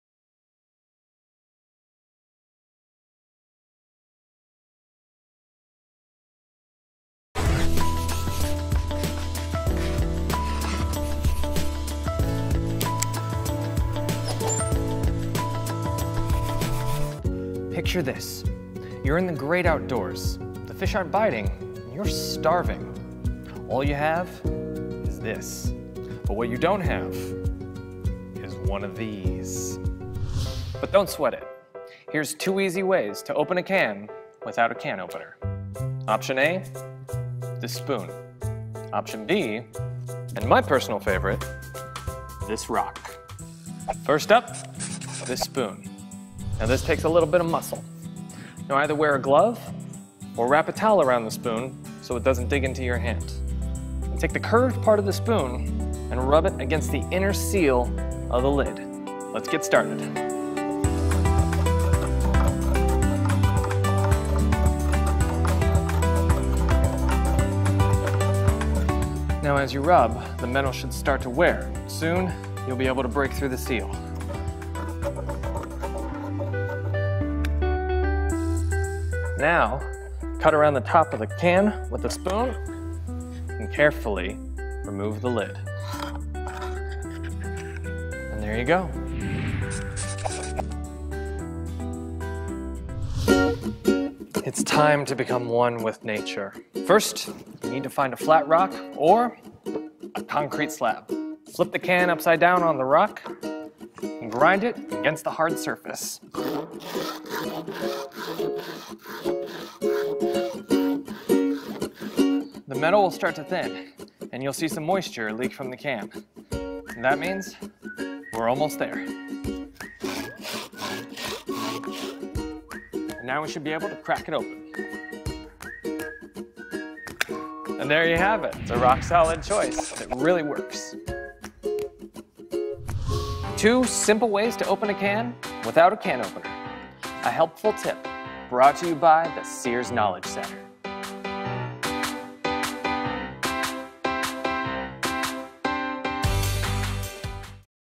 How to Open a Can Without a Can Opener
When you're at home and craving that chili in the cupboard but can't find the can opener, don't fret. Before giving in and ordering a pizza, there are a few different methods you can use to open most canned foods. Remember to use extreme caution for each of these methods as any rough cuts in the metal can be very sharp.